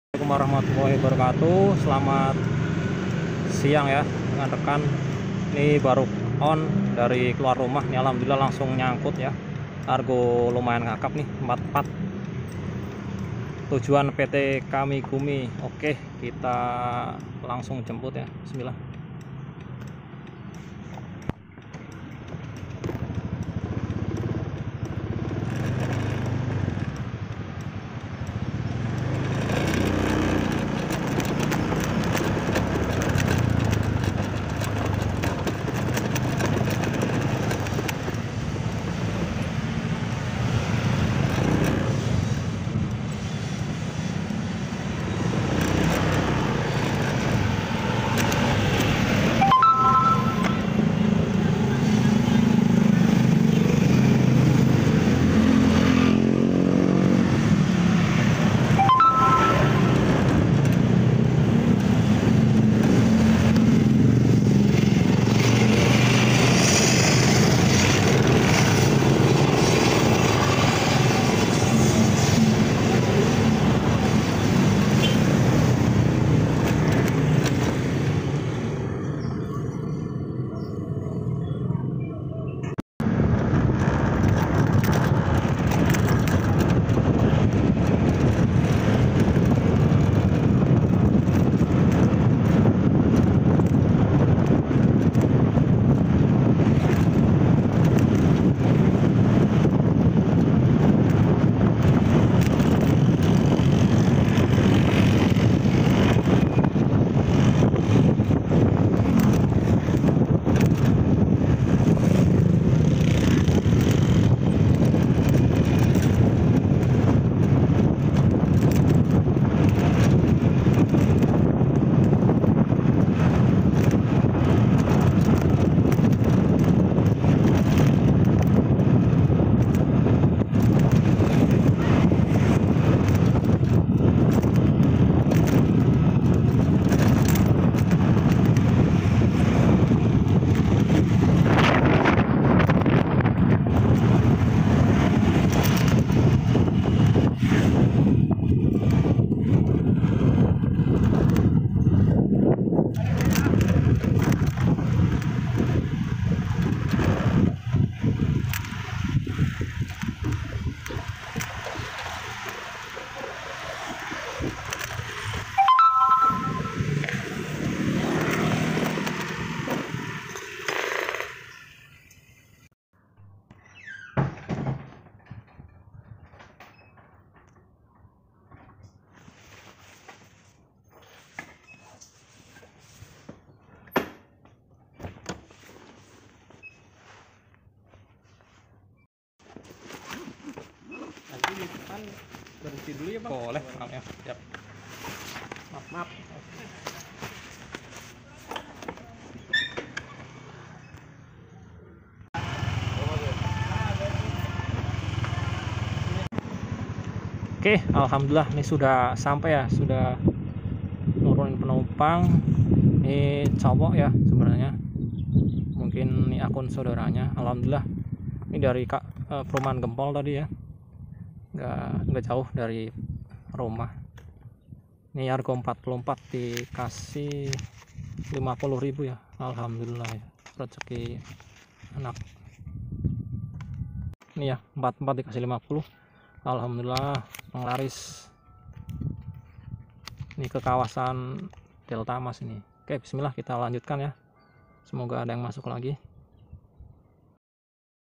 Orderan indrive Argo kakap tujuan kawasan delta mas | rezeki di hari minggu
argo 44 k dikasih 50k . terima kasih orang baik. semoga berkah.